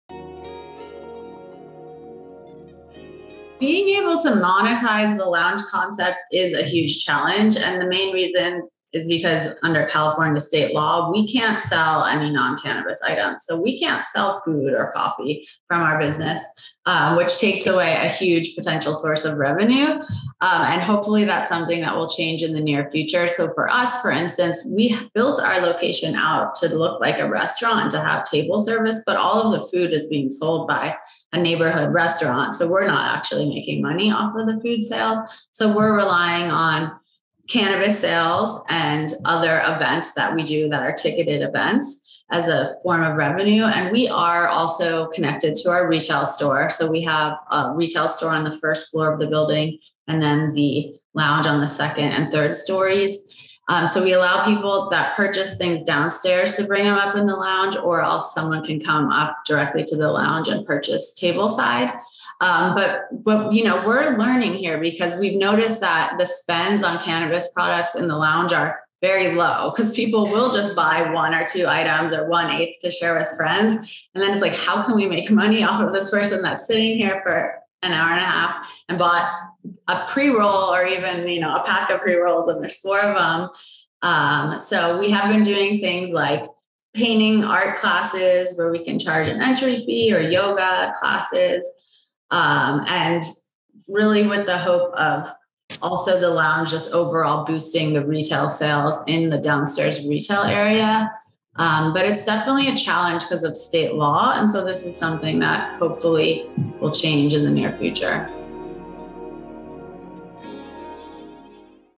Profit Model Challenges of Consumption Lounges
Consumption lounges for cannabis face many challenges when it comes to achieving profitability.
The Cannabis Chamber of Commerce hosts educational sessions and networking opportunities through our in person events, and through our virtual events.